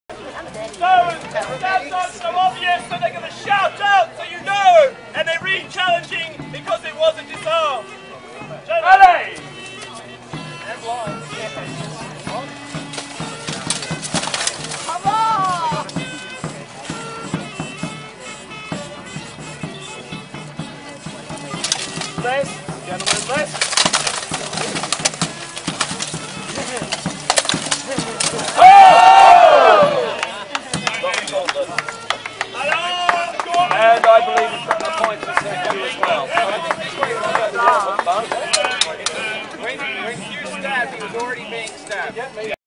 Hand to hand combat - daggers.
Footage from the 2013 Abbey Tourney.